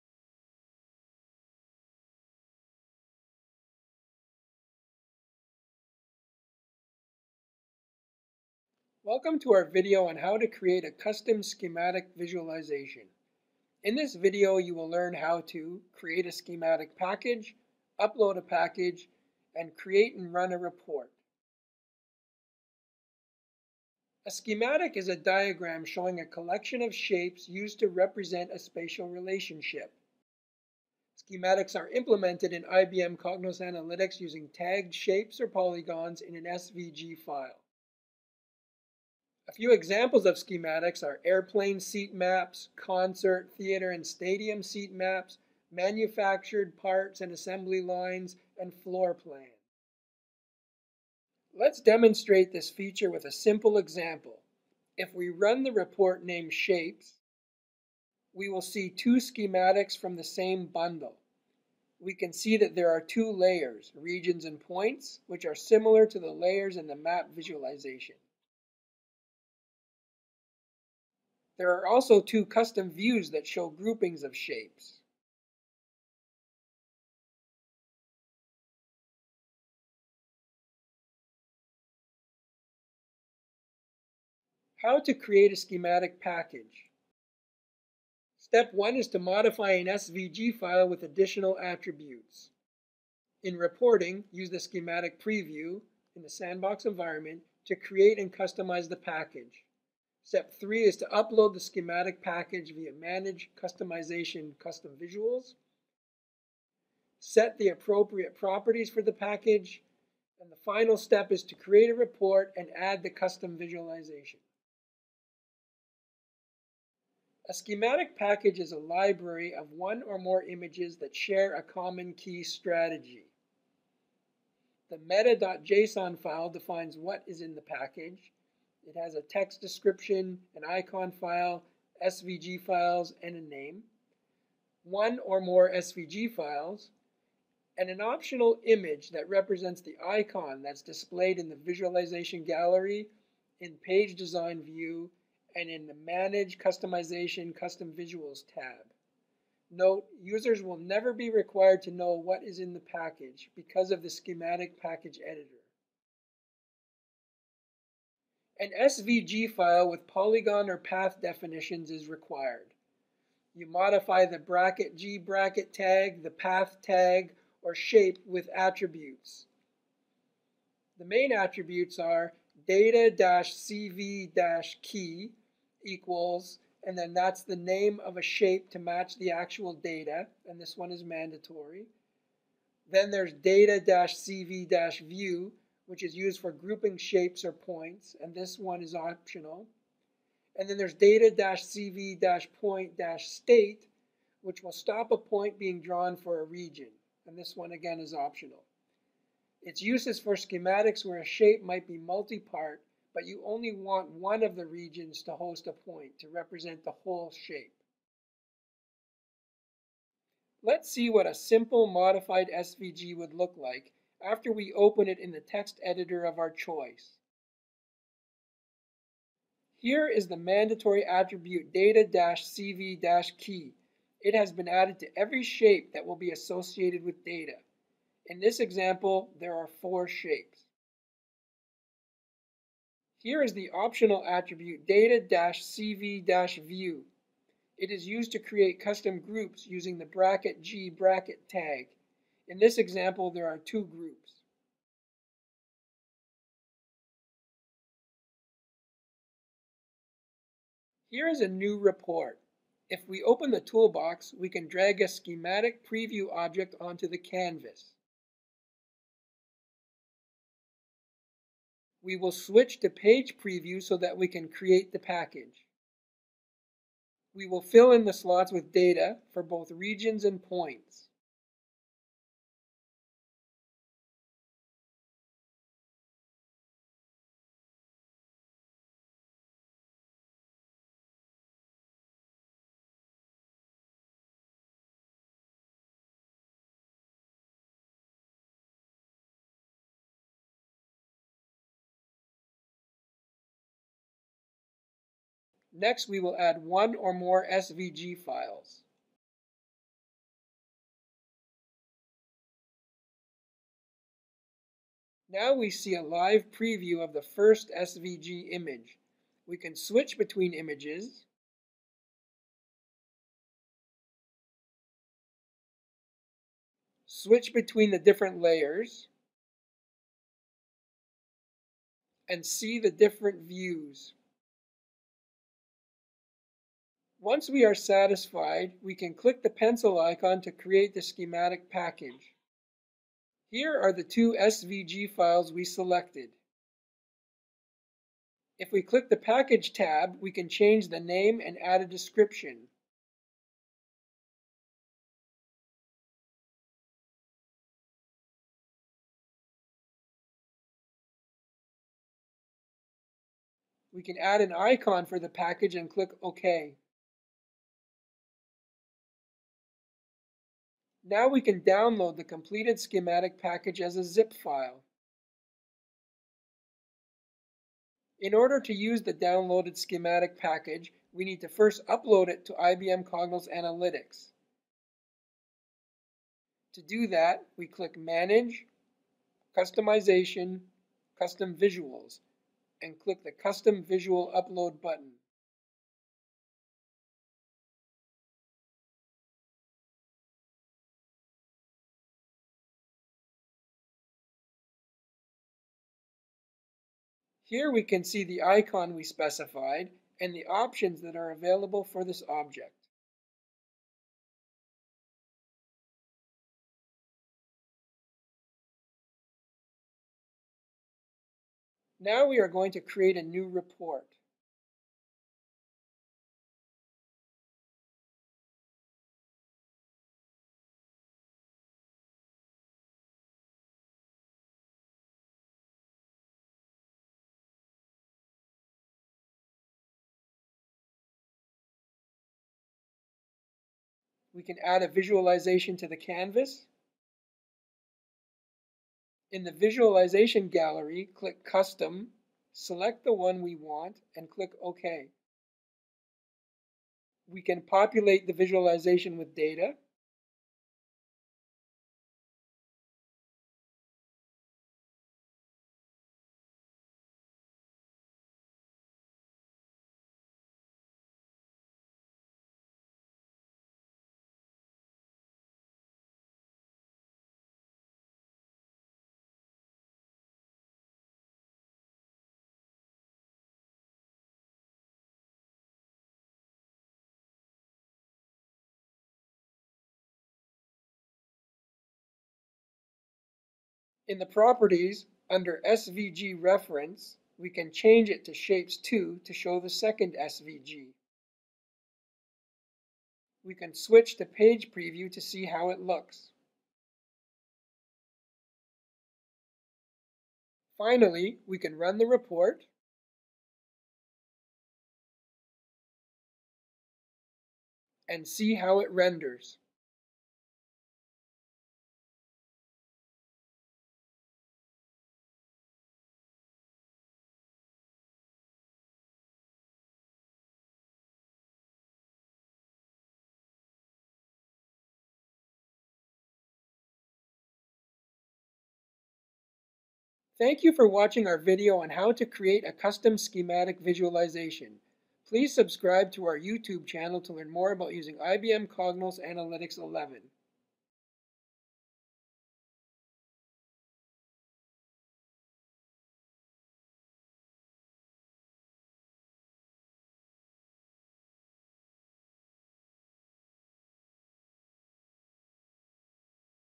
How to create a custom schematic visualization (11.1.5+)
In this video you will see a simple example of a custom schematic visualization and learn how to build and use one yourself.